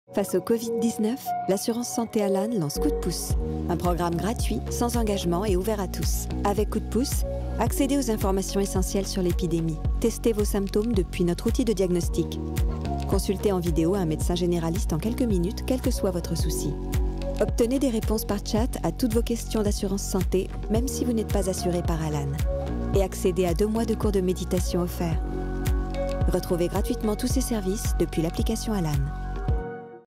Alan vous donne un coup de pouce face au COVID-19 👍
Face au COVID-19, l'assurance santé Alan lance Coup de Pouce ! 👍
Un programme gratuit, sans engagement et ouvert à tous.
Avec Coup de Pouce vous pouvez :

👩‍💻 Accéder aux informations essentielles sur l'épidémie
👍Tester vos symptômes depuis notre outils de diagnostic
🤒Consulter un médecin généraliste en quelques minutes, quelque soit votre soucis
🤳 Obtenir via le chat des réponses à toutes vos questions d'assurance santé, même si vous n'êtes pas assuré par Alan
🧘‍♀️ Accéder à 2 mois de cours de méditation offerts

Tout cela, depuis l'application Alan 🤳
Vous pouvez dès a présent télécharger l'application Alan juste ici 👇☺️